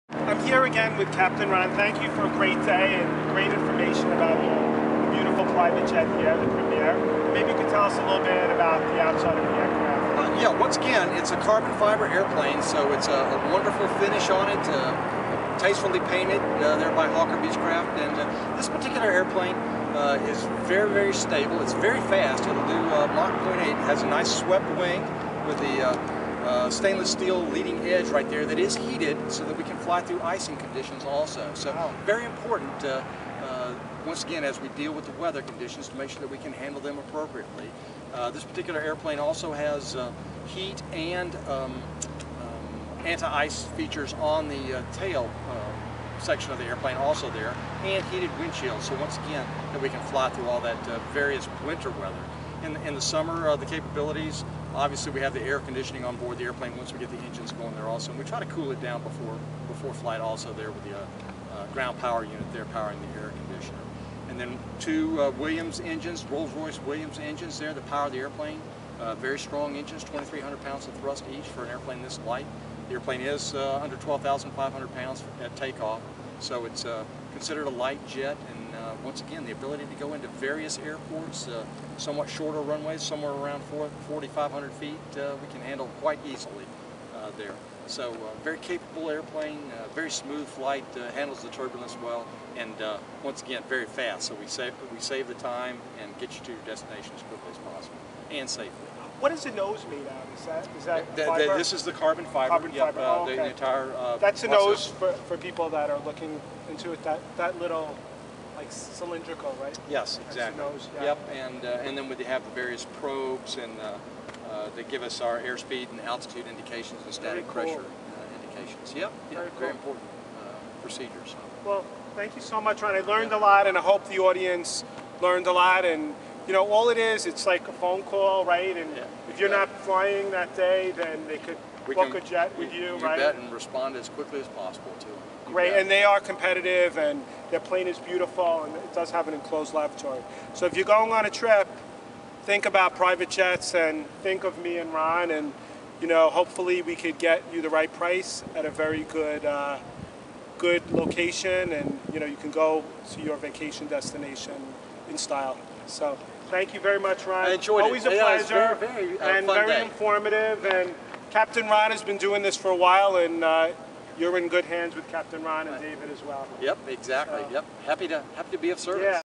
Private Jet Video
for all your jet charter needs.  This video is an interview with the captain of a light jet.  It is very informative and interesting.